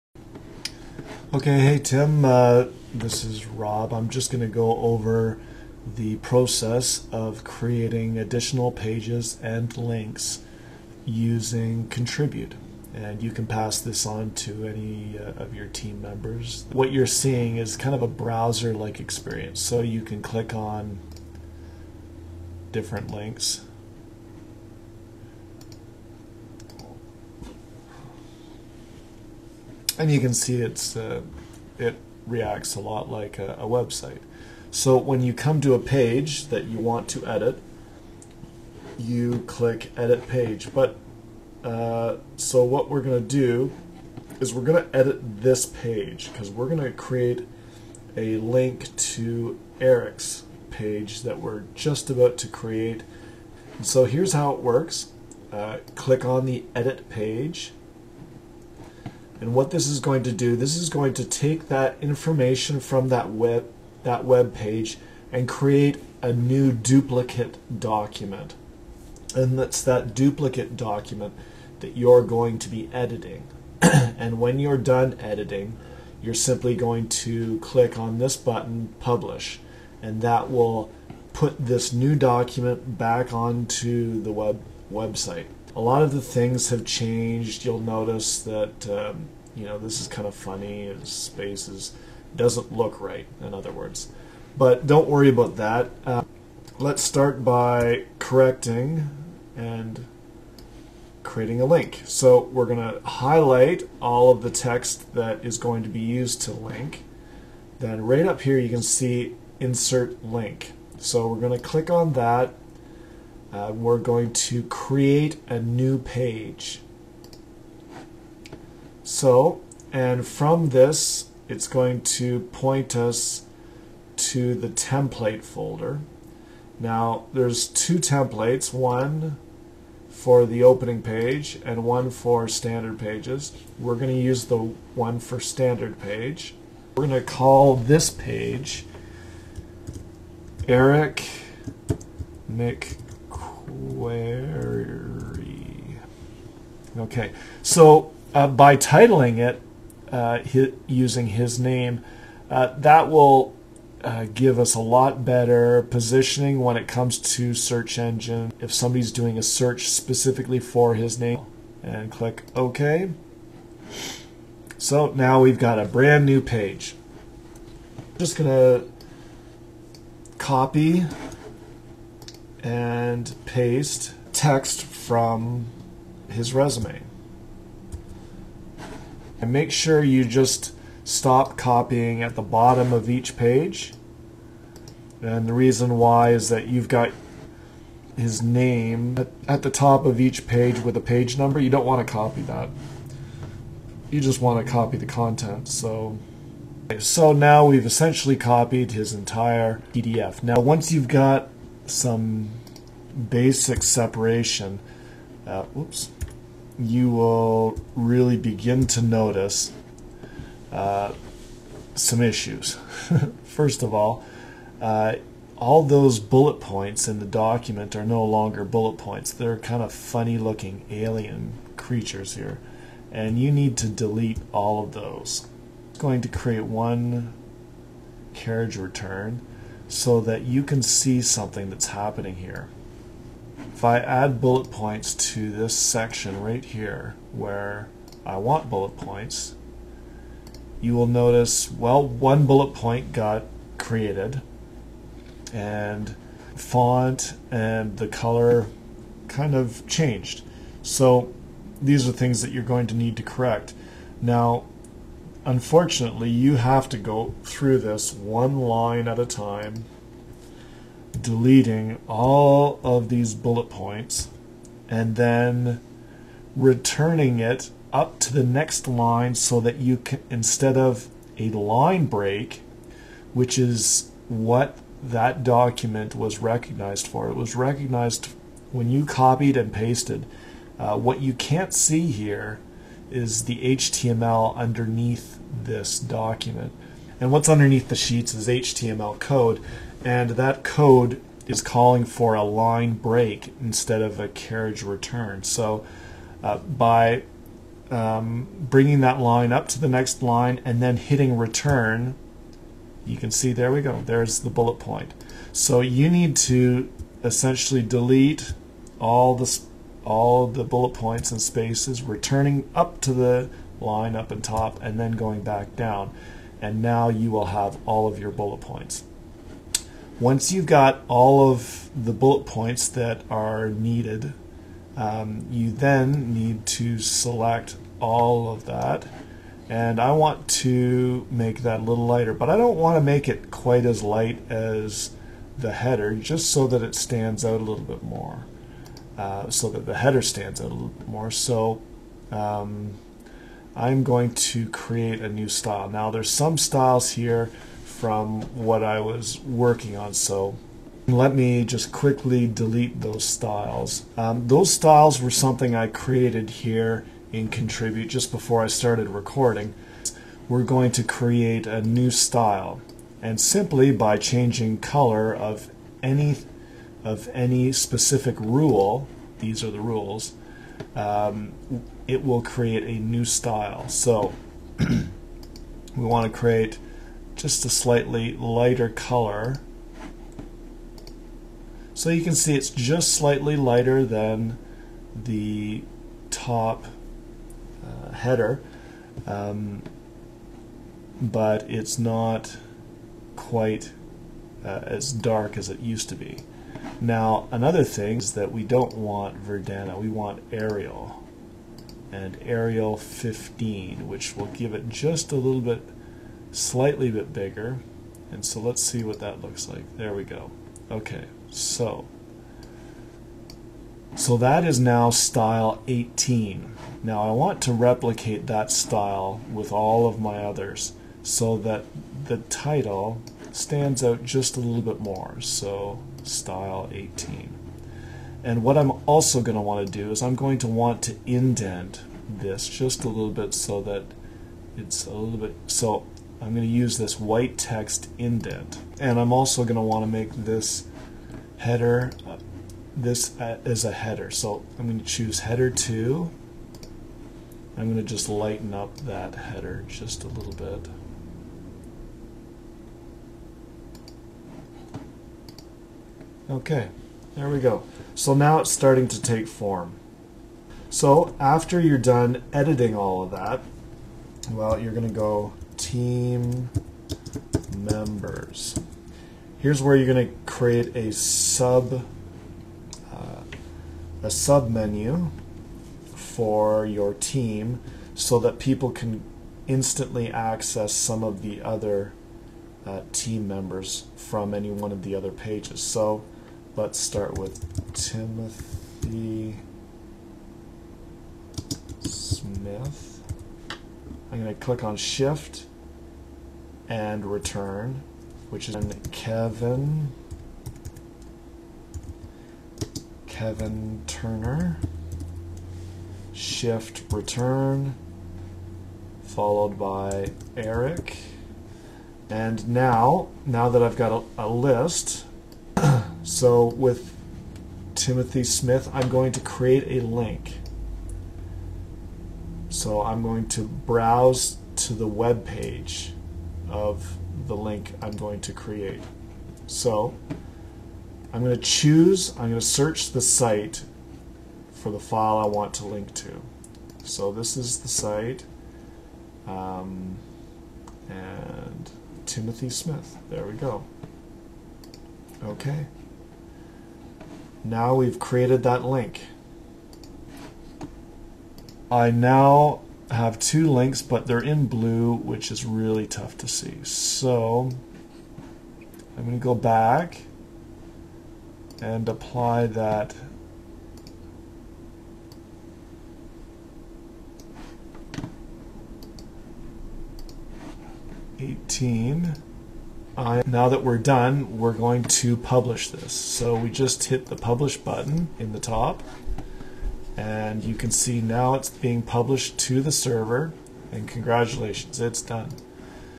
Adobe Contribute Basic Tutorial
This is the basic process of editing and creating a web page in Adobe Contribute.